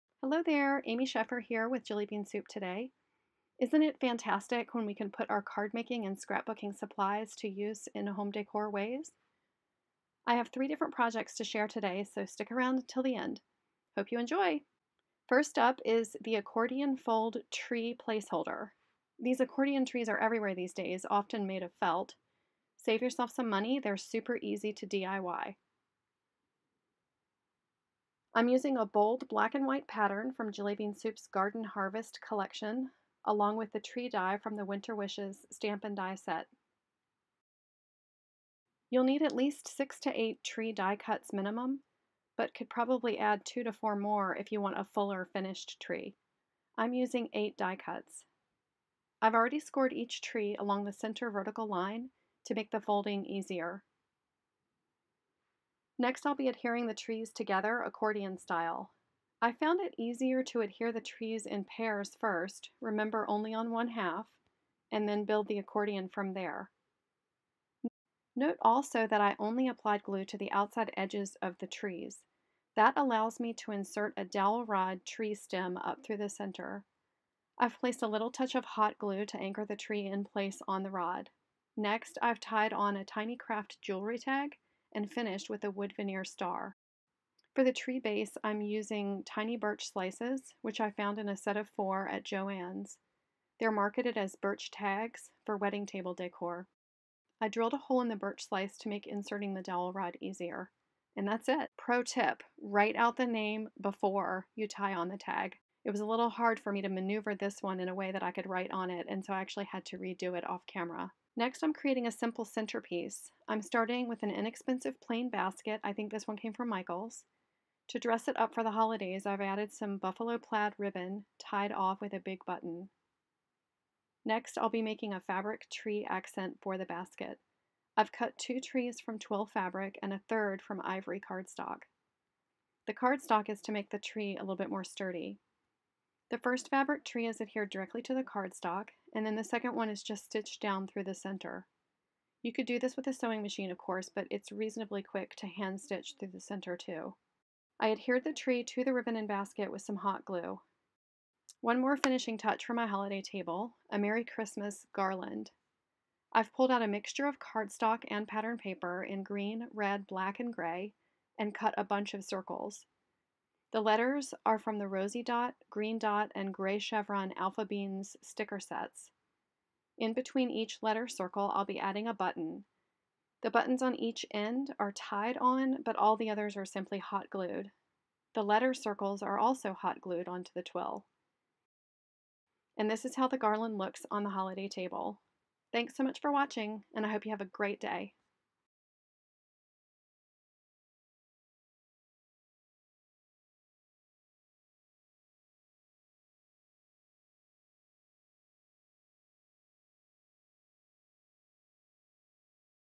Creating Beautiful Christmas Table Decorations
Join designer Amy Sheffer as she creates beautiful table decorations for your Christmas table.